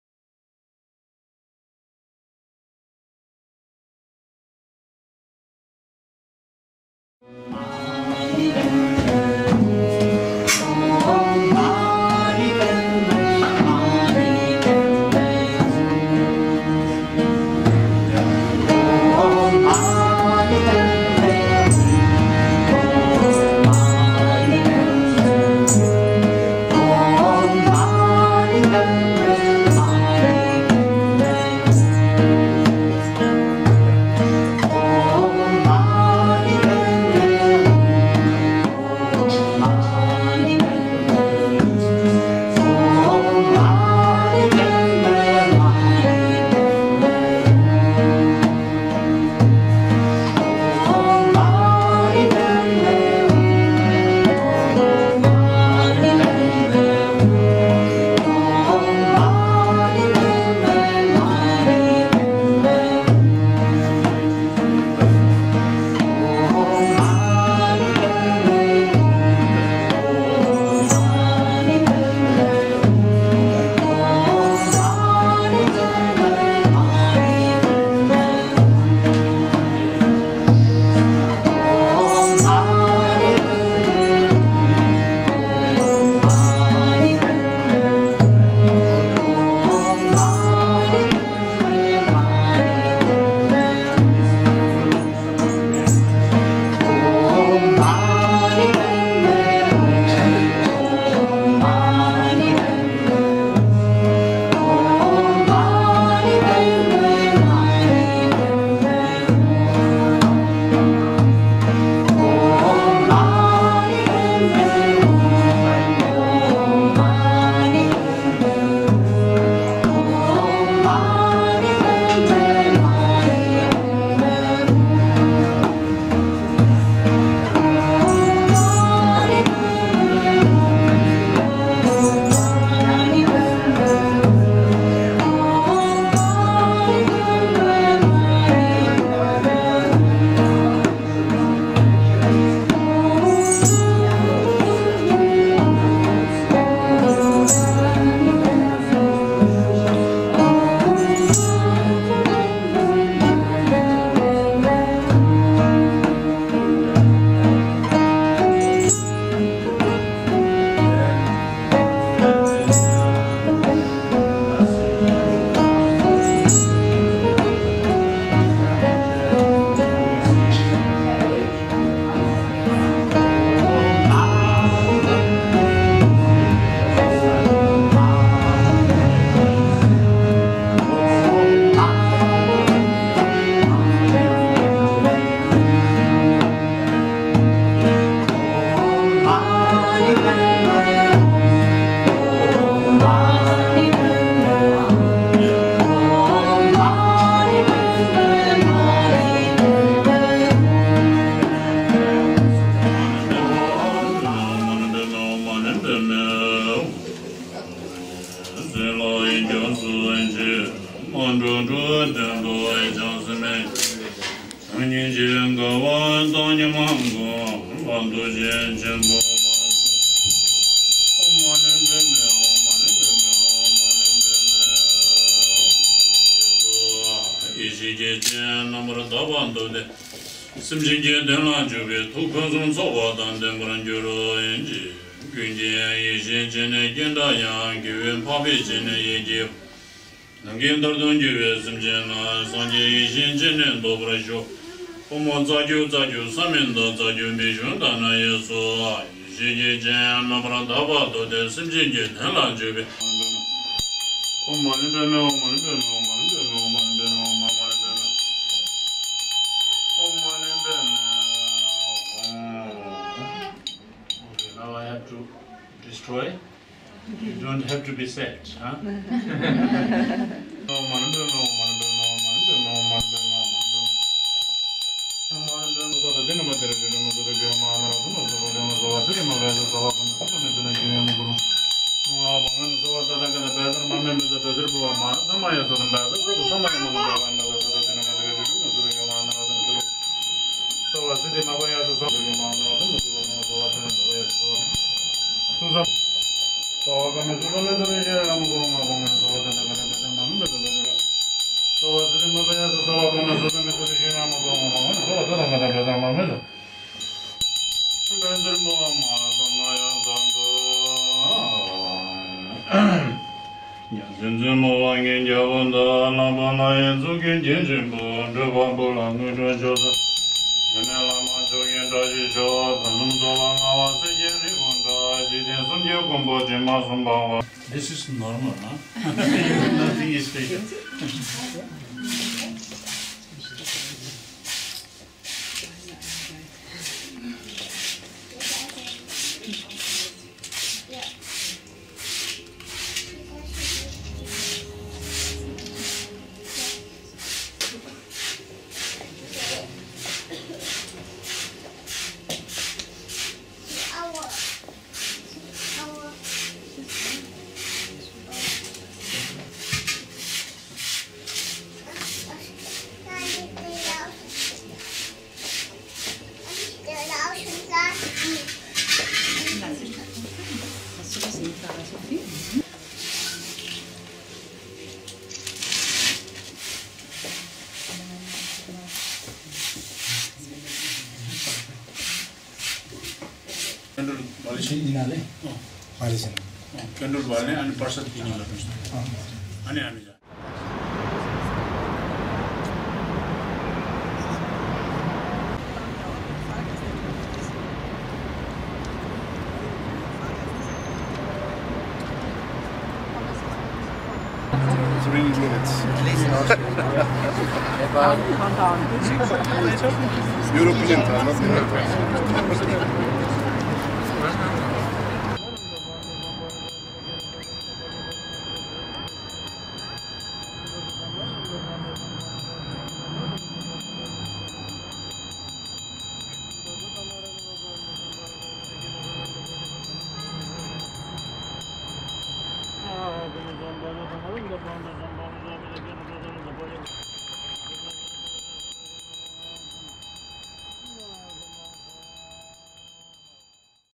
Yak+Yeti Nepal-Kulturrestaurant Sandmandala Lama Ngawang 2012
Im August 2012 streute Lama Ngawang Kunga Bista ein Mandala im YAK+YETI.
Der Erlös diente zur Förderung der Schule in seinem Dorf Lo-Manthang.